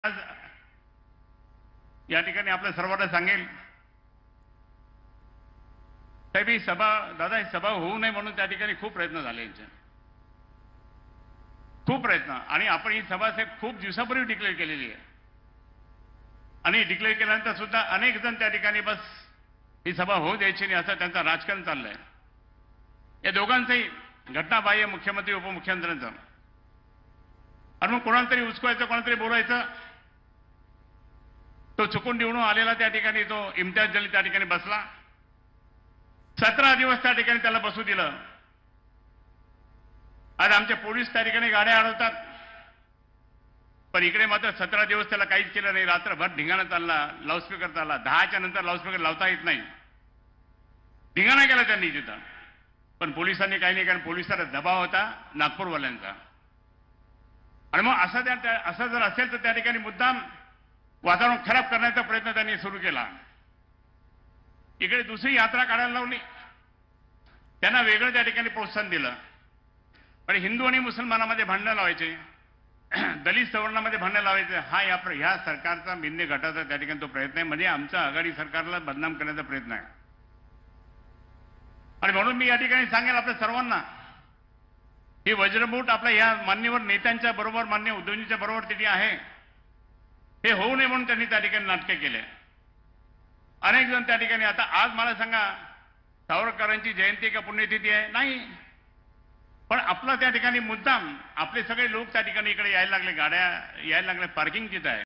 Chandrakant Khaire MVA Sabha : हिंदू-मस्लिम, दलित-सवर्ण भांडणं लावायची हा सरकारचा प्रयत्न
आजची सभा होऊ नये यासाठी पोलिसांवर नागपूरचा दबाव असल्याचा आरोप शिवसेना ठाकरे गटाचे नेते चंद्रकांत खैरे यांनी केला. शिवसेना फोडण्याचं काम फडणवीसांनी केलं असल्याचं तानाजी सावंत यांनी सांगितलं. सावरकरांना भारतरत्न देत नाही, पण त्यांच्या नावाने यात्रा काढता असा आरोप त्यांनी शिंदे-फडणवीसांवर केला.